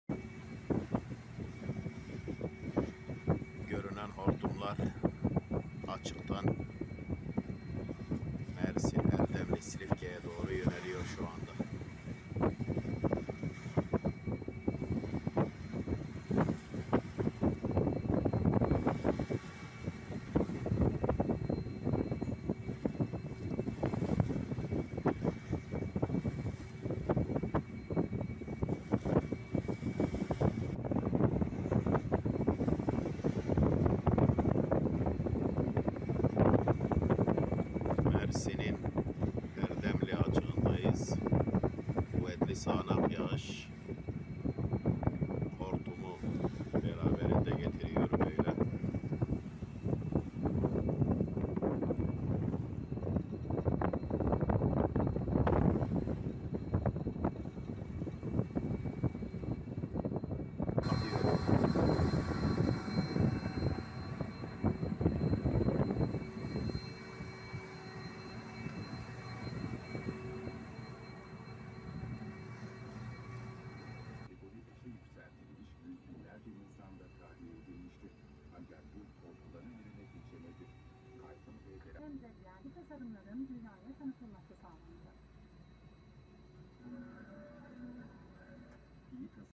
Denizi bulutlar kapladı hortum çıktı
Mersin’de Doğu Akdeniz'in açıklarını bulutlar kaplarken devasa bir de hortum çıktı. O anlar bir balıkçı kaptanı tarafından görüntülendi.

Sabah saatlerinde Mersin’in Erdemli ve Silifke ilçelerinin 12 mil açığının bulutlarla kaplanmasının ardından yağışla birlikte hortum çıktı. Çıkan hortum kilometrelerce uzaktan görüntülenmesine rağmen büyüklüğü dikkat çekti. Zaman zaman rüzgarın etkili olduğu denizde, hortum yaklaşık 15 dakika sürdü. İlçelerin kıyısına doğru yönelen hortumun etkisini kaybetmesi nedeniyle olumsuz bir durum yaşanmadı.